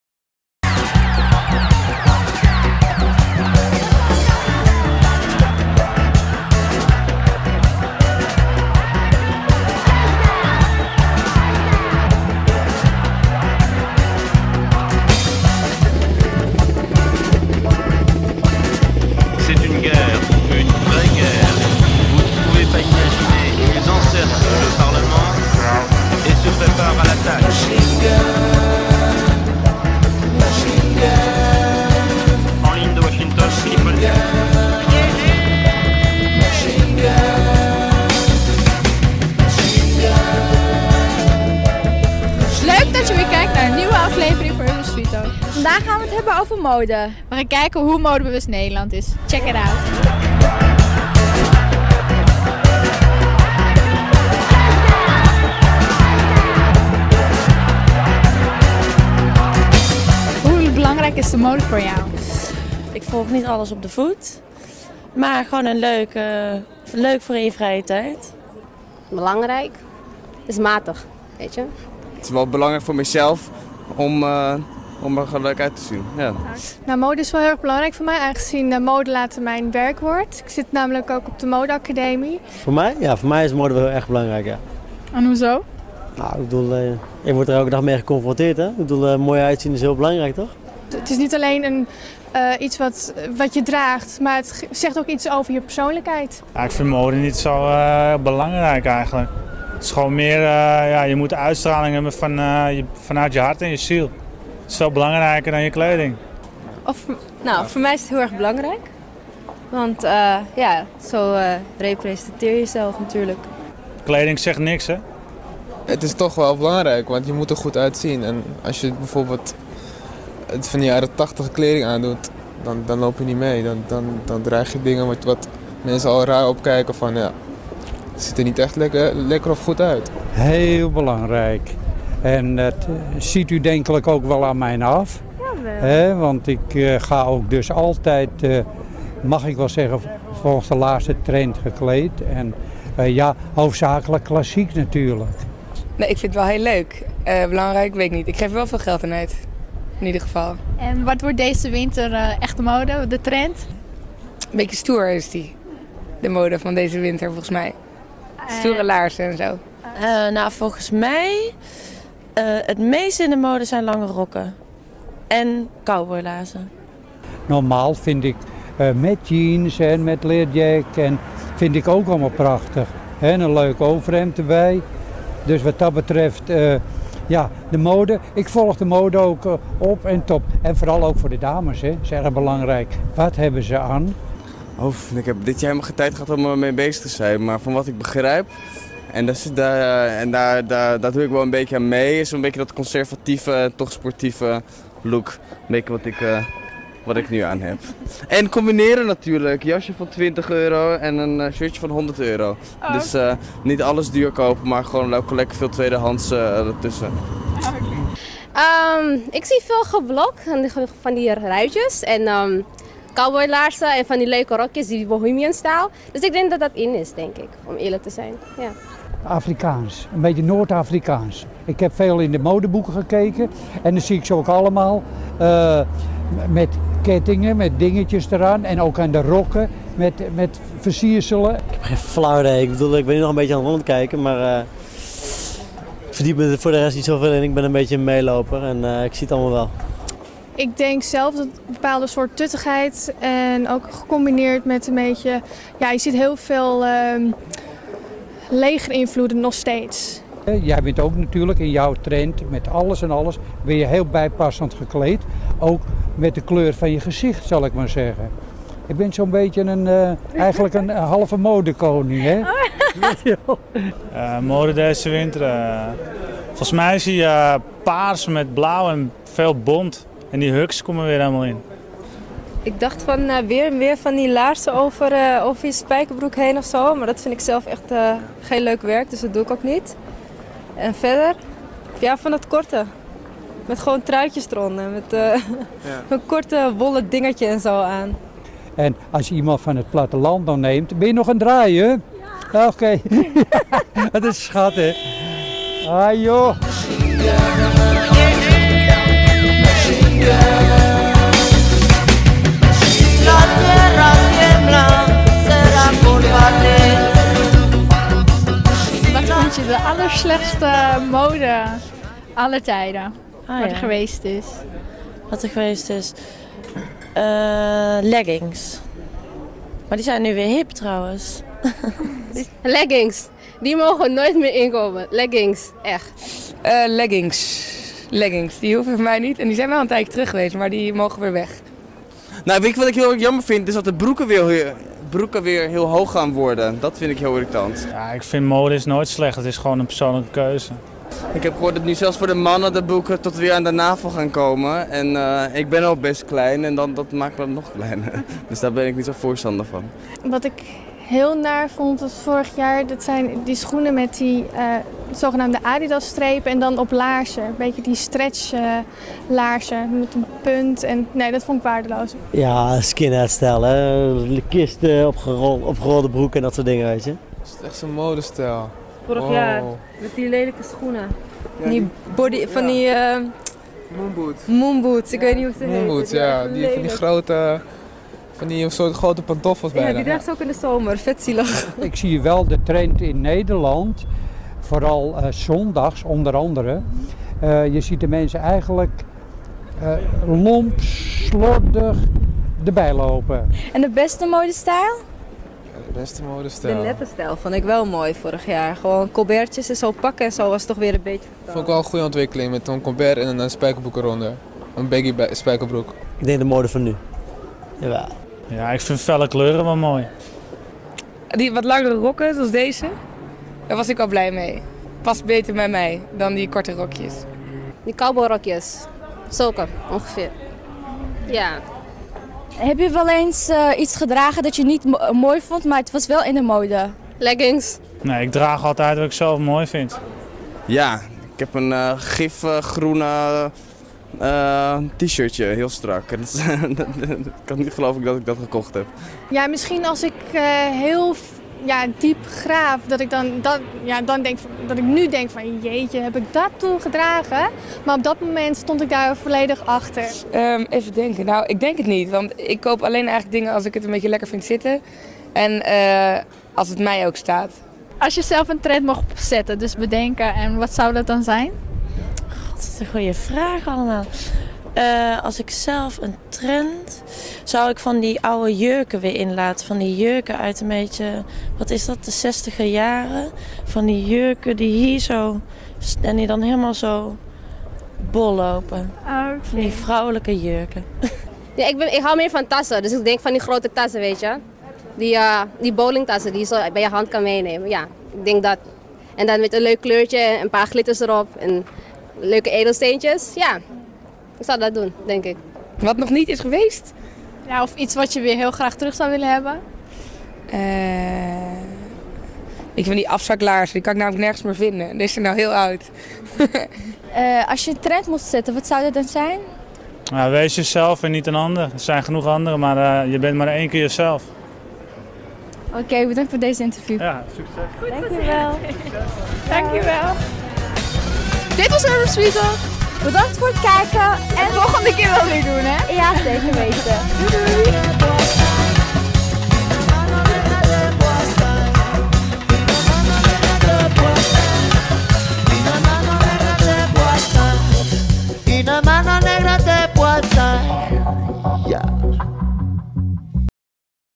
URBAN STREETTALK - MODE
Wat kan wel en wat kan echt niet meer in de mode? Diana en Samira vragen het aan Amsterdammers op straat.